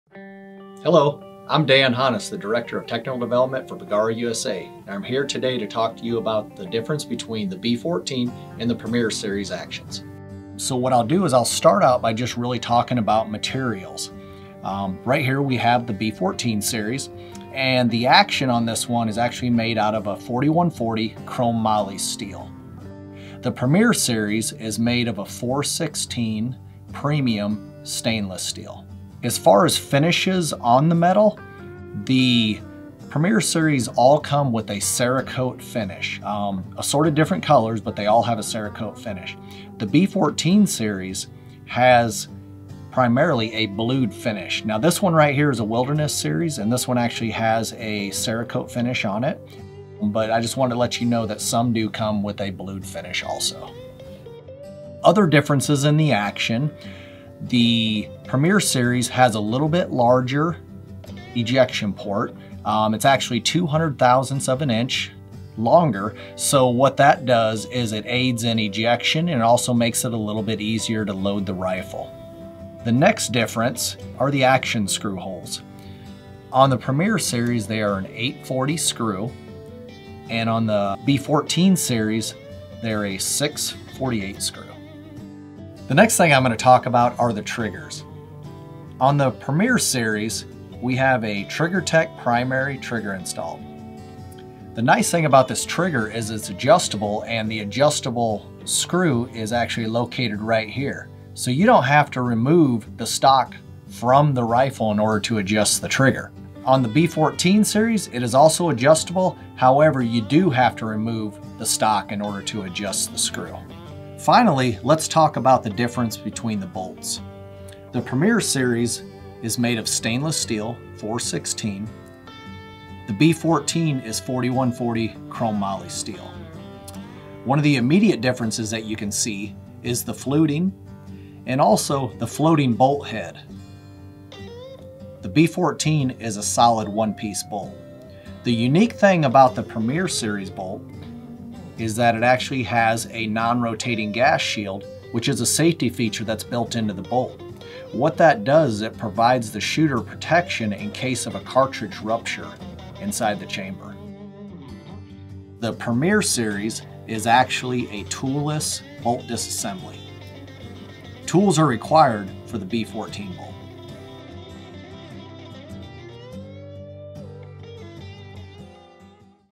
Descubre las diferencias entre la serie B-14 y Premier de Bergara Rifles
Dan Hanus, nuestro director técnico de Bergara USA compara y contrasta las series B-14 y Premier.

SERIE B-14
- 4140 Cromoly Acción de acero, cerrojo y cañón.
- Normalmente el acabado es pavonado excepto en la serie Wilderness que es Cerakote
- 6-48 con agujeros roscados para las bases REM 700
- El disparador Bergara Performance de ajuste interno
- Un cerrojo de una pieza

SERIE PREMIER
- 416 premios Acero Inox. Acción, cerrojo y cañón de acero inoxidable de primera calidad
- Acabado en Cerakote
- Un puerto de expulsión más grande para facilitar la carga y la expulsión
- 8-40 agujeros roscados para las bases del telescopio Remington 700
- TriggerTech trigger (ajustable externamente)
- Cerrojo acanalado con escudo de gas no giratorio
- El cerrojo tiene un desmontaje sin herramientas

¡Dispara con nosotros!